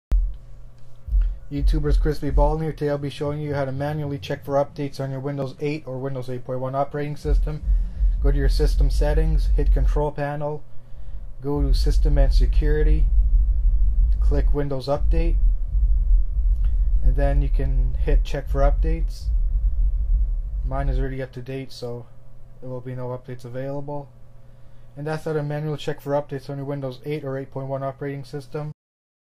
Manually Check For Windows Updates on Windows 8 or 8.1 OS
This is probably by far the most easiest and quickest way to check for windows updates on your Windows 8 & 8.1 OS. Tutorial.